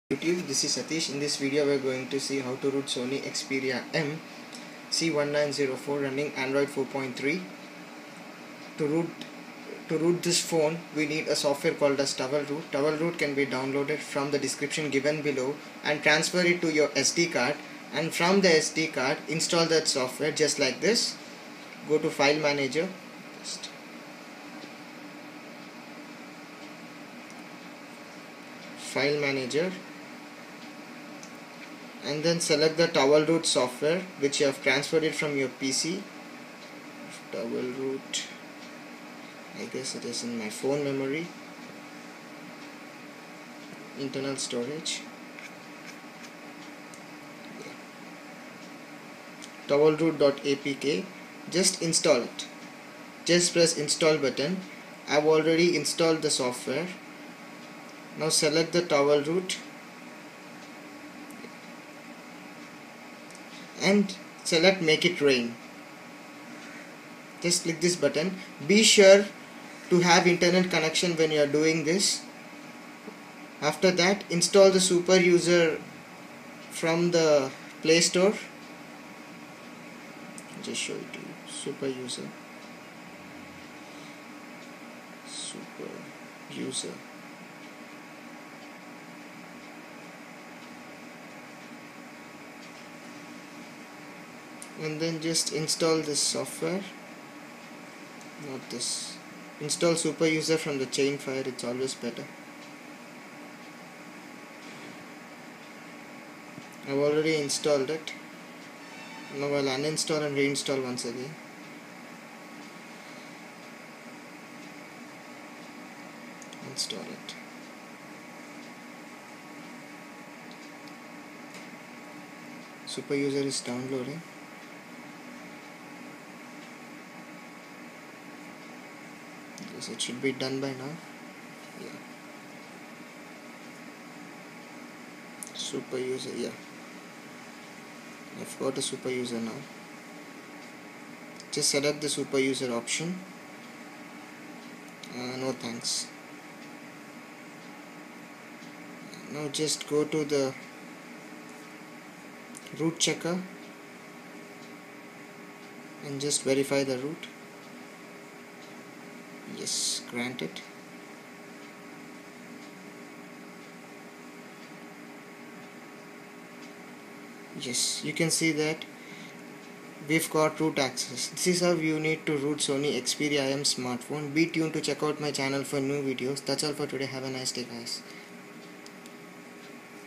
How to root sony xperia m running 4.3
This is method to root any android phone running android jellybean and above. The link to towel root is given below

Thanks to geohot for making this root.
thumbs up if yo like the video.